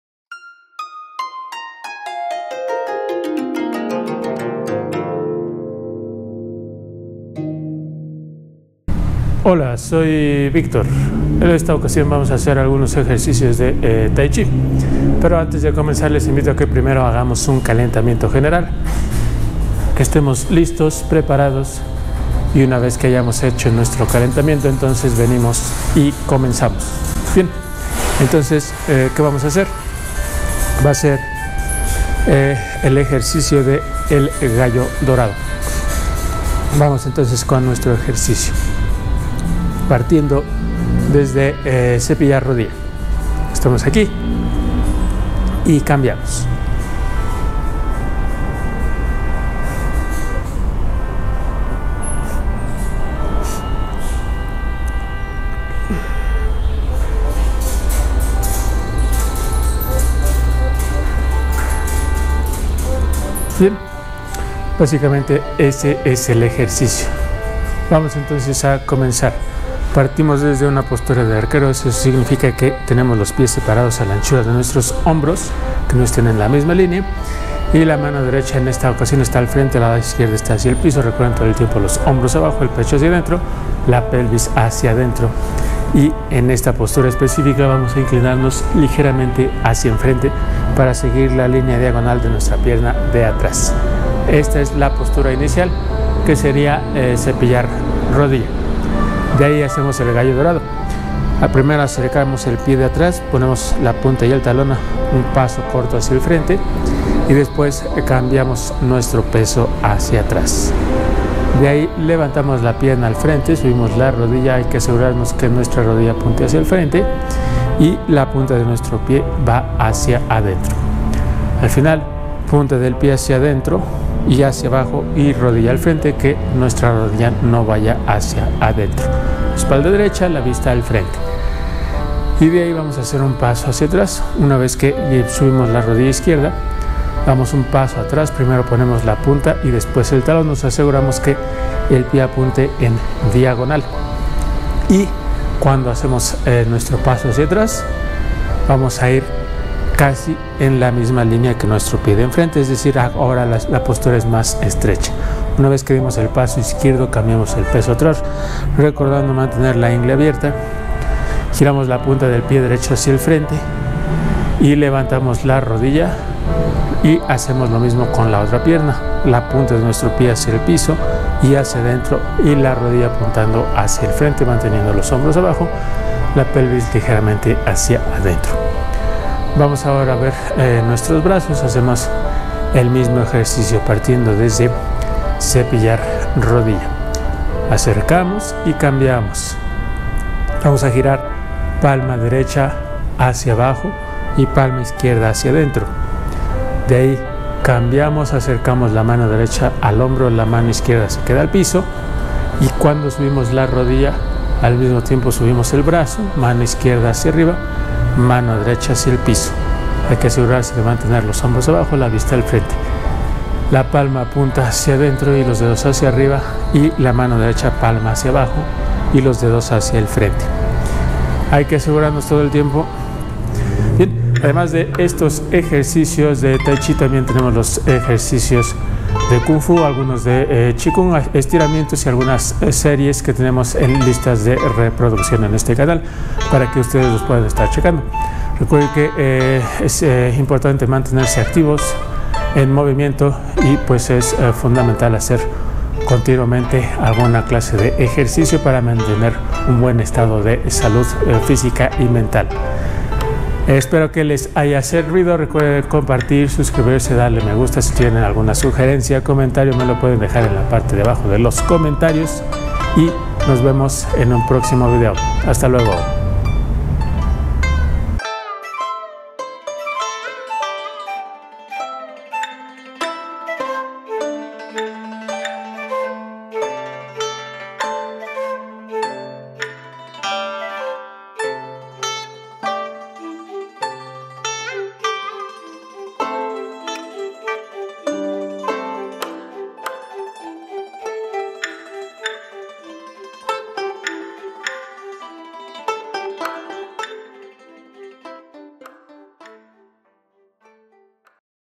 El gallo dorado desde cepillar rodilla 1
En este video realizamos el ejercicio de el gallo dorado desde cepillar rodilla.